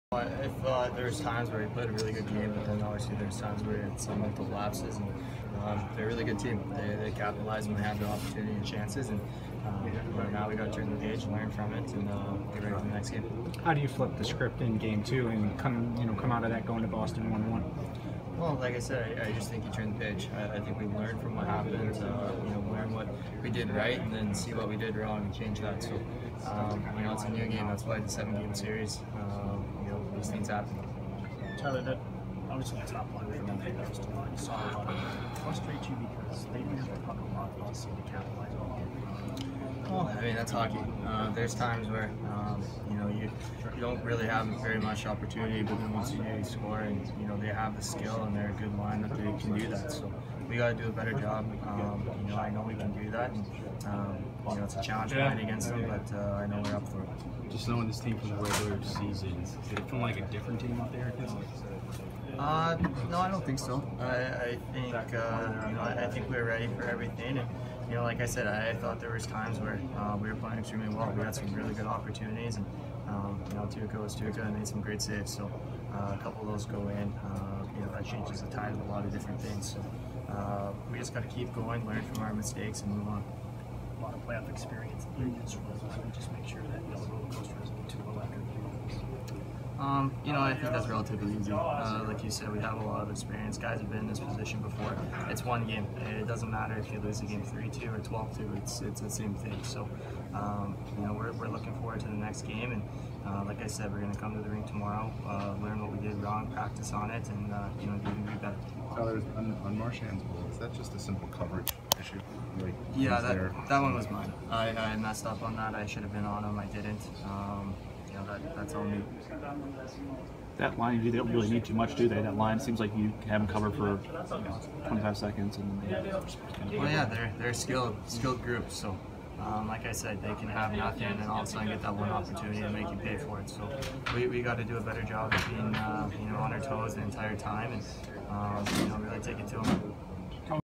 Tyler Johnson Round 2, Game 1
Lightning forward Tyler Johnson talks to the media after the Lightning’s 6-2 Game 1 loss